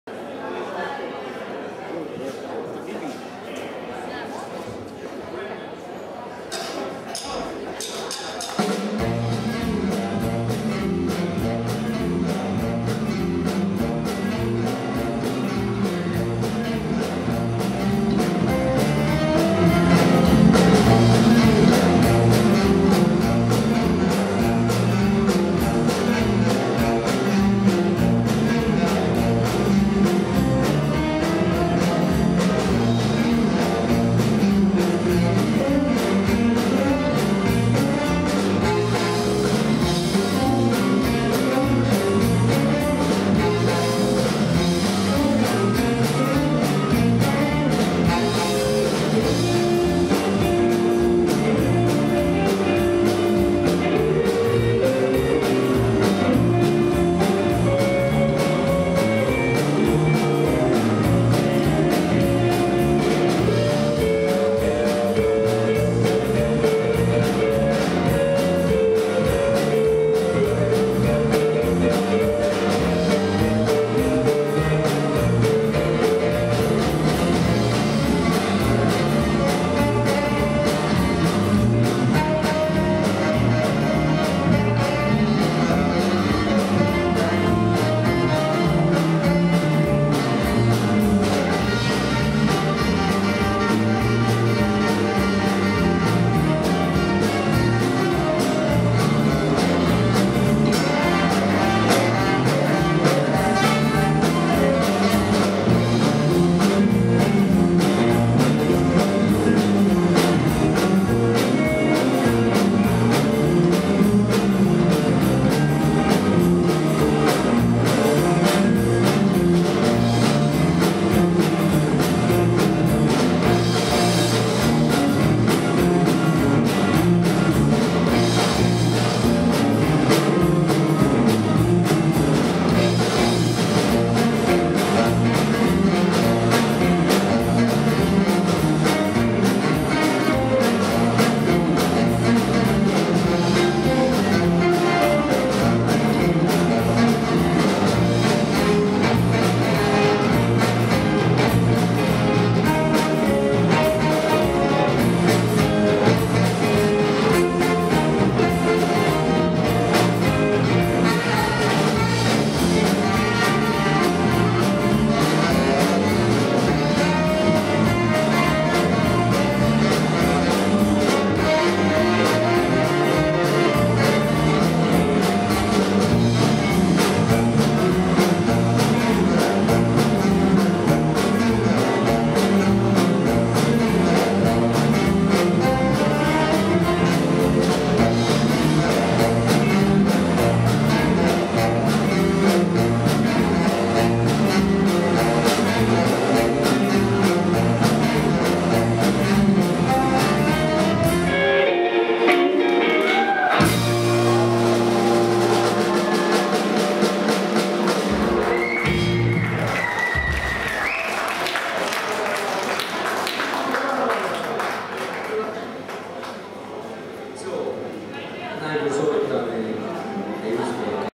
Blue Jam Ribbon plays at Tallinn Rock Cafe 2005
BJR from Finland plays Tallinn Rock Cafe restaurant in 2005.